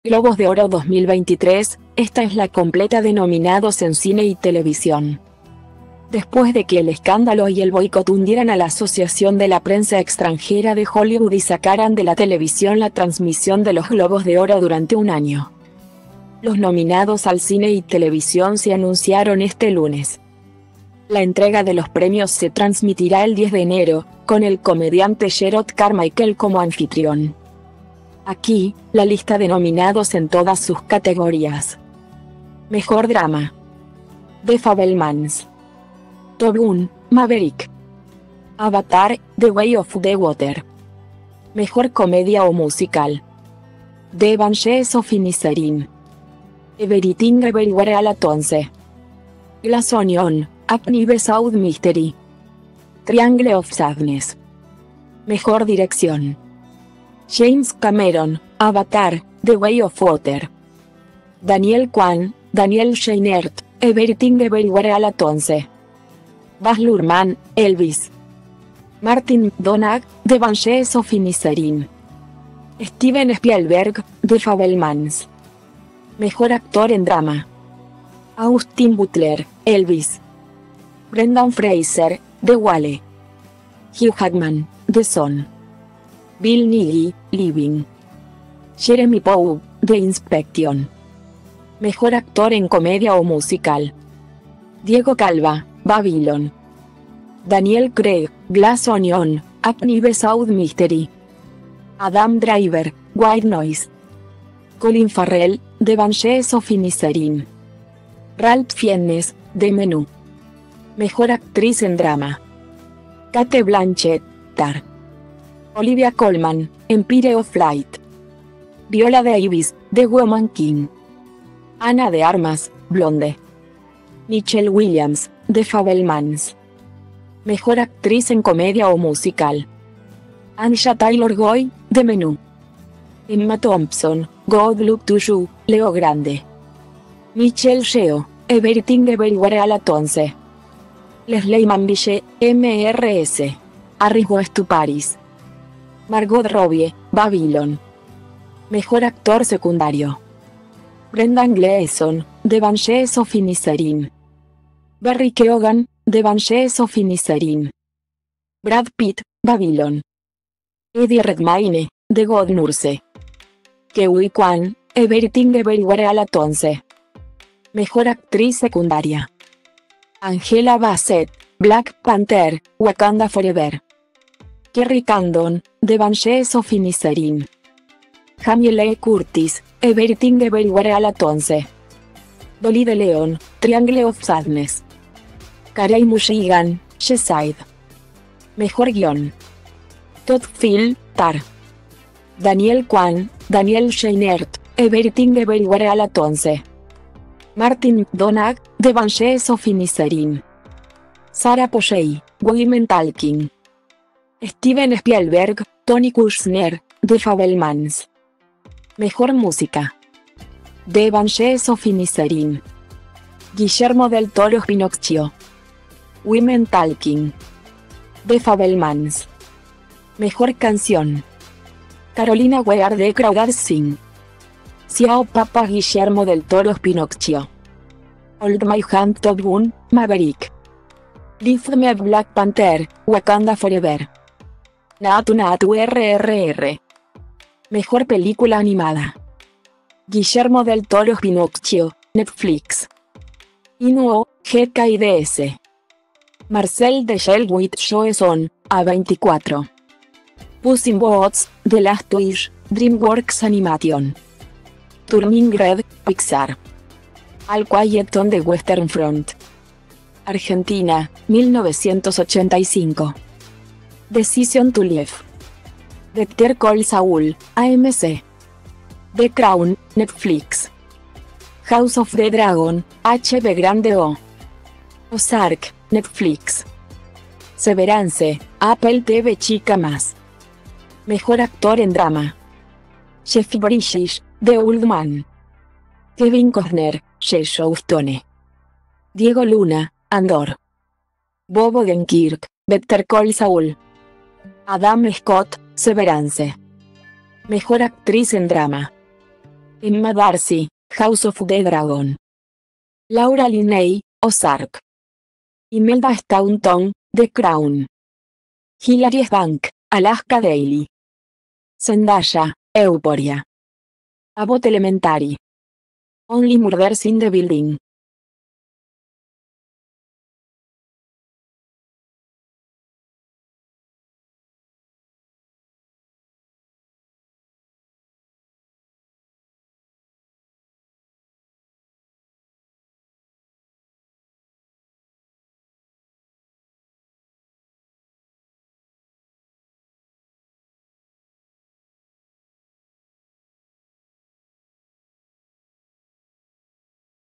Globos de Oro 2023: esta es la completa de nominados en cine y TV
Después de que el escándalo y el boicot hundieran a la Asociación de la Prensa Extranjera de Hollywood y sacaran de la televisión la transmisión de los Globos de Oro durante un año, los nominados al cine y televisión se anunciaron este lunes. La entrega de los premios se transmitirá el 10 de enero, con el comediante Jerrod Carmichael como anfitrión.